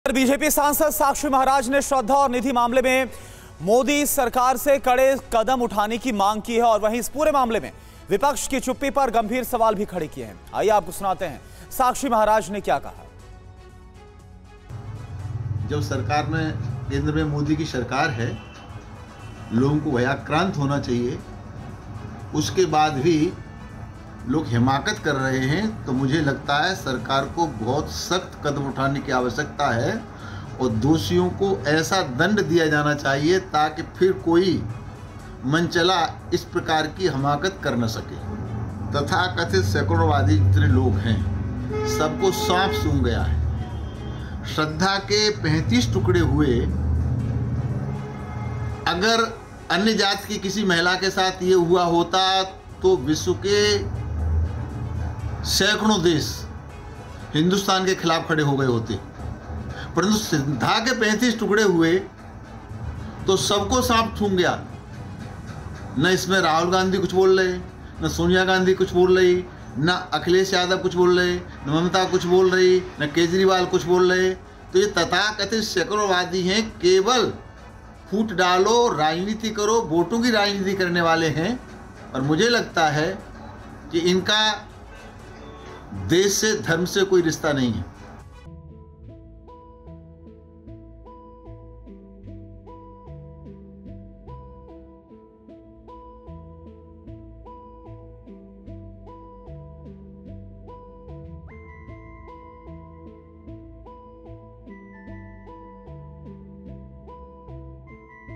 Shraddha Murder Case: BJP सांसद Sakshi Maharaj ने विपक्ष की चुप्पी पर खड़े किए गंभीर सवाल
बीजेपी सांसद साक्षी महाराज ने श्रद्धा और निधि मामले में मोदी सरकार से कड़े कदम उठाने की मांग की है..वहीं इस मामले में विपक्ष की चुप्पी पर गंभीर सवाल खड़े किए हैं...आइए सुनते हैं साक्षी महाराज ने क्या कहा है